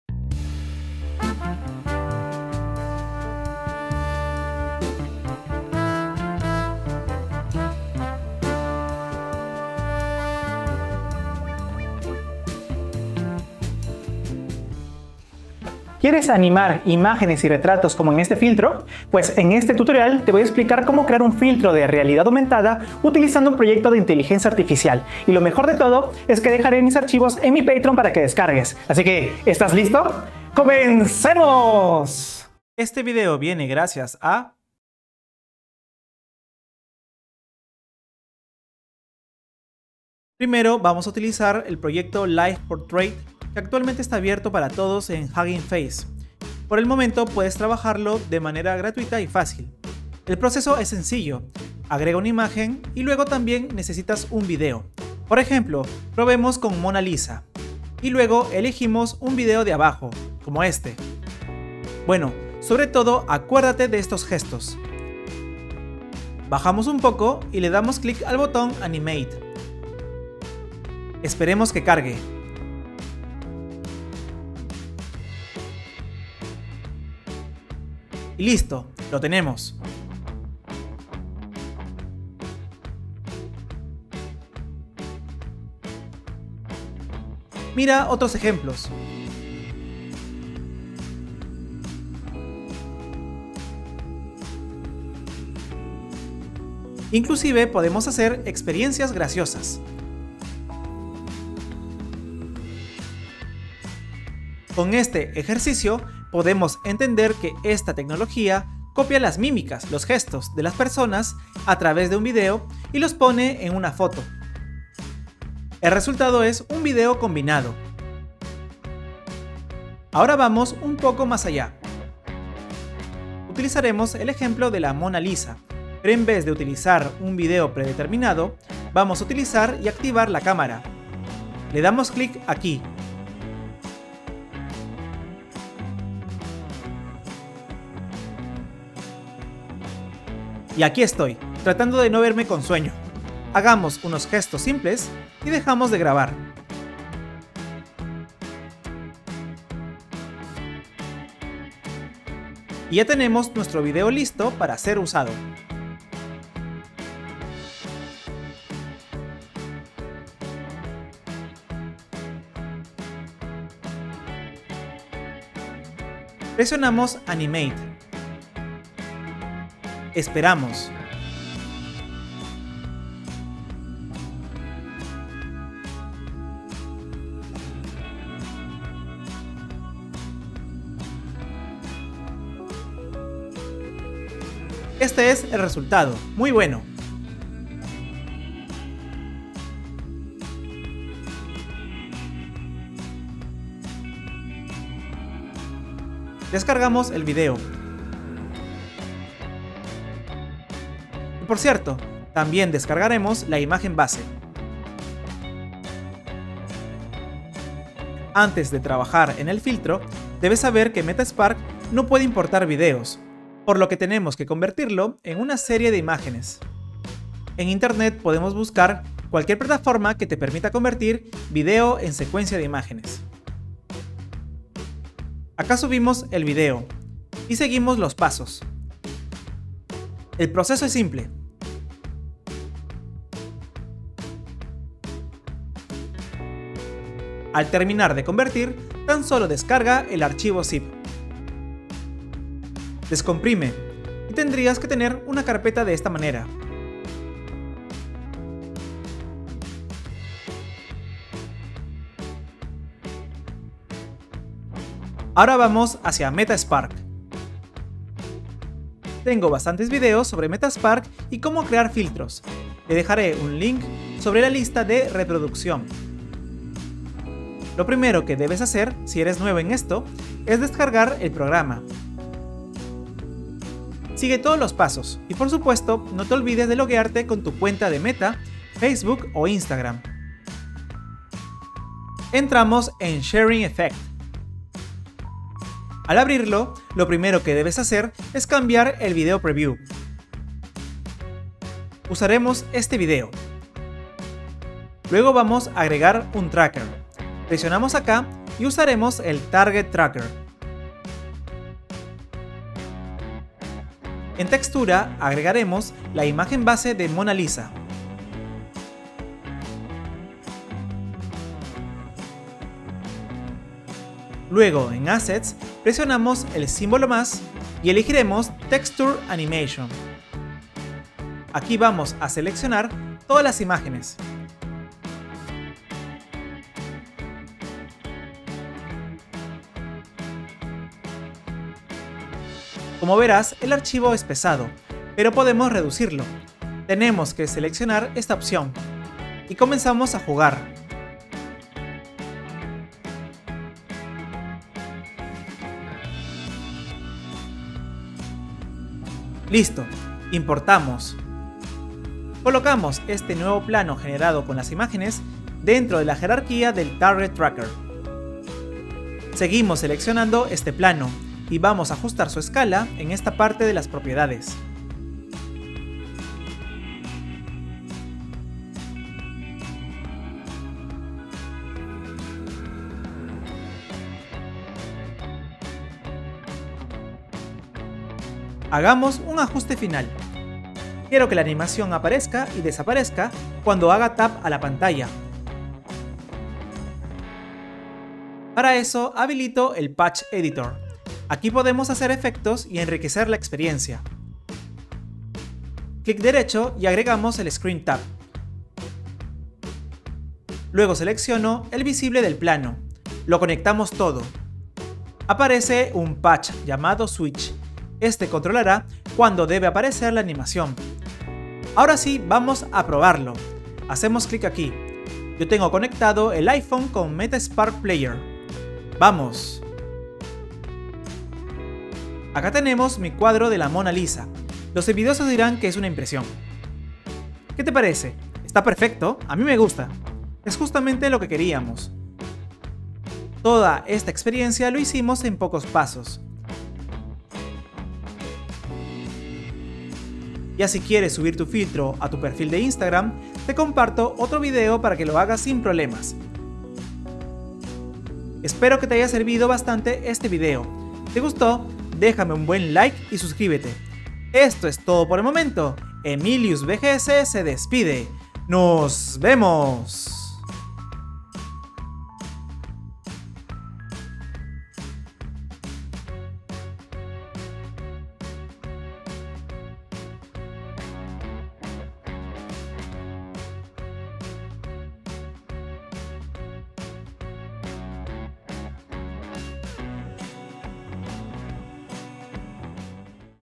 Anima imágenes y retratos con Live Portrait y Meta Spark | Filtro de realidad aumentada
Crea animaciones realistas de tus retratos y personajes a través del proyecto de inteligencia artificial llamado LIVE PORTRAIT y la plataforma de realidad aumentada Meta Spark. Vamos a crear un potente filtro de instagram
Create realistic animations of your portraits and characters through the artificial intelligence project called LIVE PORTRAIT and the augmented reality platform Meta Spark. Let's create a powerful instagram filter

Mi VR / AR software y hardware:
⚡ Oculus Quest
⚡ Aryzon
⚡ Zapbox
⚡ Merge Cube